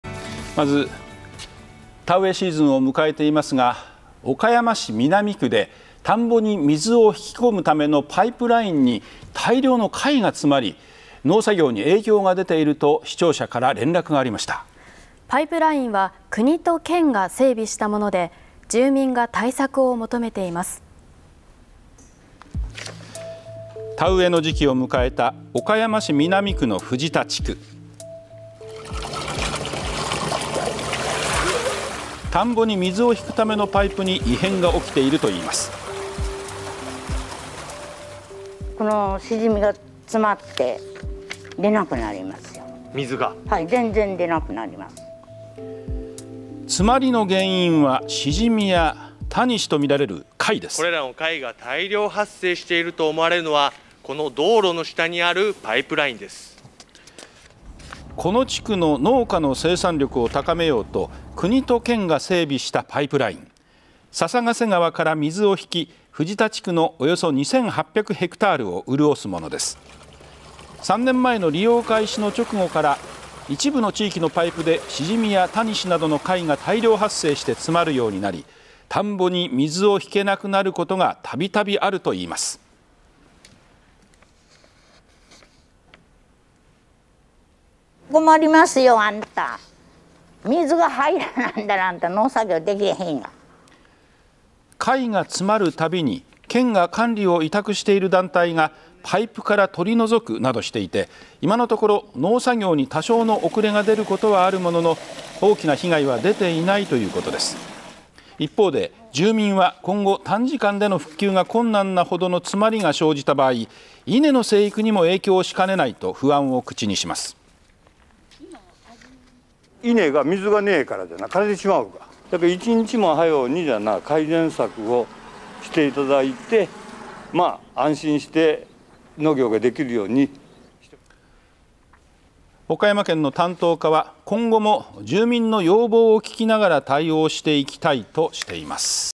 「田んぼの水がピンチ」岡山・藤田地区に農業用水を引き込むパイプを詰まらせたのは、貝！？
岡山県南では田植えシーズンを迎えていますが、岡山市南区で田んぼに水を引き込むためのパイプラインに大量の貝が詰まり、農作業に影響が出ていると視聴者から連絡がありました。